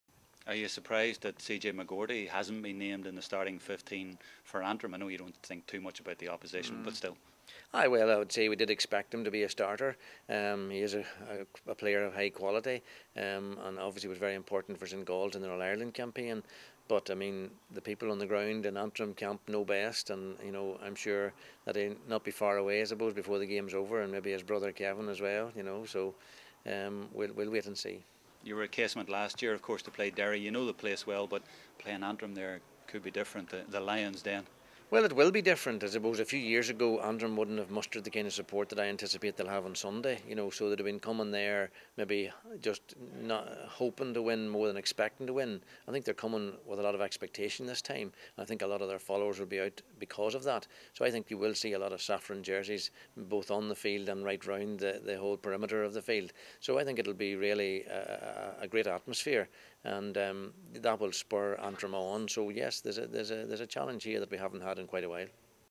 No CJ for Antrim
CJ McGourty is a surprise omission from Antrim's starting 15 for the Ulster SFC Quarter-final with Tyrone, but Mickey Harte expects to see him play at some stage in Casement Park.